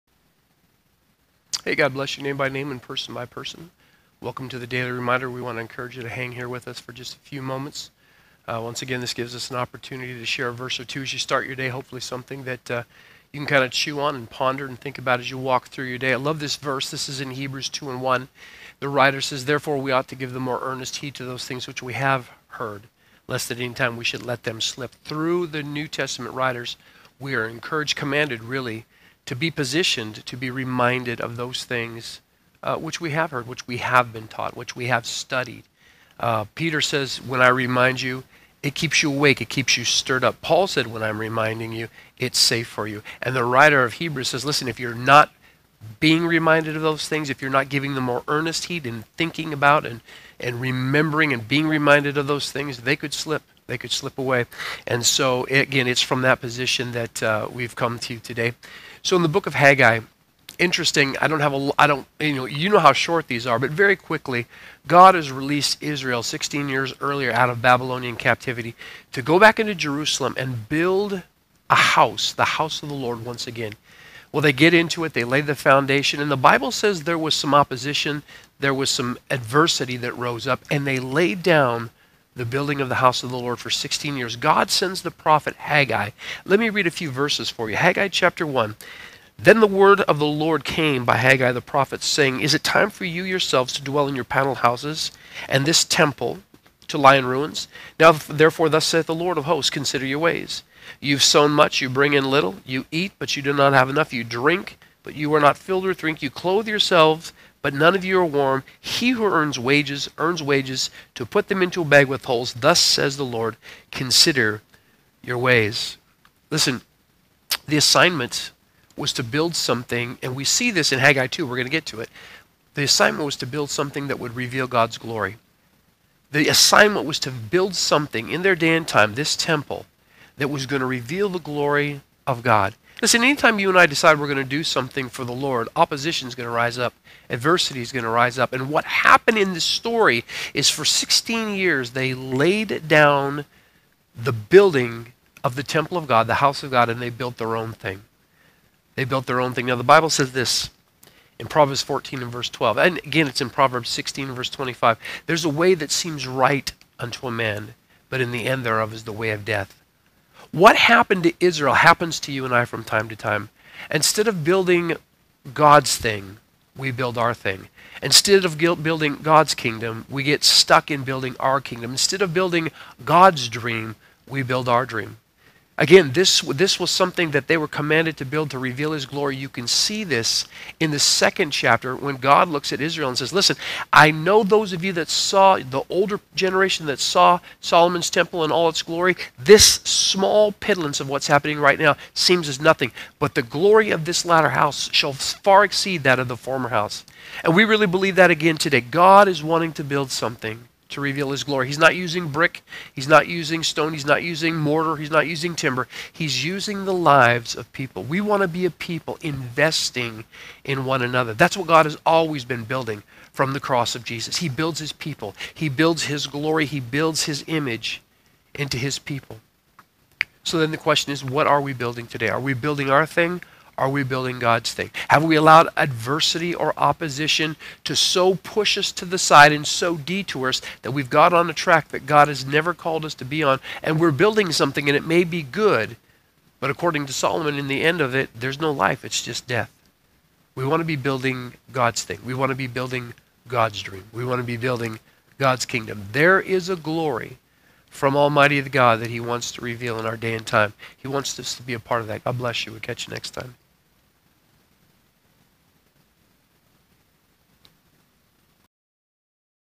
"GOD BUILDS THE HOUSE"
"GOD BUILDS THE HOUSE"  (4:00)
Pastor Scott Olin's Daily Reminder - 9-03-20

New Hope City Church, Longview, wa
Declaring God's Word, bringing New Hope!